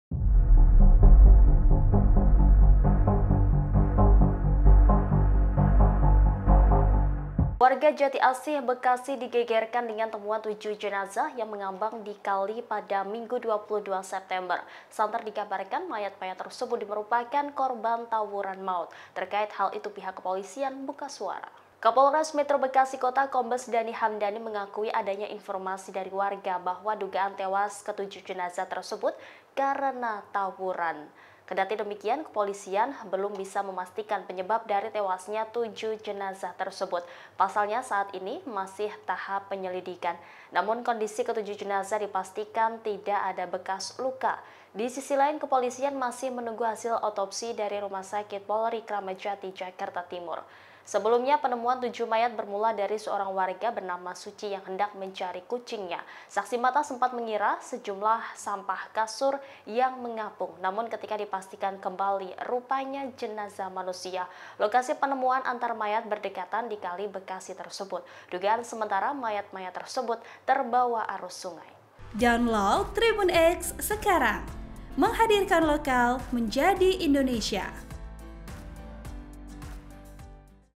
Geger 7 Mayat Mengambang di Kali Bekasi Diduga Korban Tawuran, Polisi: Kondisi Tidak Ada Bekas Luka
TRIBUN-VIDEO.COM - Warga Jatiasih, Bekasi digegerkan dengan temuan 7 jenazah yang mengambang di kali pada Minggu (22/9/2024).

Santer dikabarkan mayat-mayat tersebut merupakan korban tawuran maut.

Terkait hal itu polisi buka suara.

Kapolres Metro Bekasi Kota, Kombes Dani Hamdani mengakui adanya informasi dari warga bahwa dugaan tewas ketujuh jenazah tersebut karena tawuran.

“Informasi dari warga karena tawuran, tapi perlu kami konfirmasi juga dugaan itu, dan kami belum bisa pastikan,” kata Kombes Dani Hamdani, Minggu, 22 September 2024.

Kendati demikian kepolisian belum bisa memastikan penyebab dari tewasnya 7 jenazah tersebut.

Pasalnya saat ini masih tahap penyelidikan.

Namun, kondisi ketujuh jenazah dipastikan tidak ada bekas luka.

“Kalau kondisi jenazah tidak ada bekas luka,” jelasnya.

Di sisi lain, kepolisian masih menunggu hasil autopsi dari Rumah Sakit Polri Kramat Jati, Jakarta Timur.

“Selanjutnya jenazah masih diperiksa di RS Polri guna keperluan penyelidikan lebih lanjut, terkhusus juga identitas para jenazah,” tutup Kombes Dani Hamdani.

Sebelumnya, penemuan 7 mayat bermula dari seorang warga, Suci (42) yang hendak mencari kucingnya.

Saksi mata sempat mengira sejumlah sampah kasur yang mengapung.

Namun ketika dipastikan kembali rupanya jenazah manusia.

Lokasi penemuan antar mayat berdekatan di kali Bekasi tersebut.

Dugaan sementara mayat-mayat tersebut terbawa arus sungai.

(Tribun-Video.com)

Artikel ini telah tayang di Tribunbekasi.com dengan judul Santer Dikabarkan Korban Tawuran, Polisi Belum Pastikan Penyebab Tujuh Jasad di Kali Bekasi,

Program: Tribunnews Update
Host: Rima Anggi
Editor Video: Mellinia Pranandari
Uploader: Tri Hantoro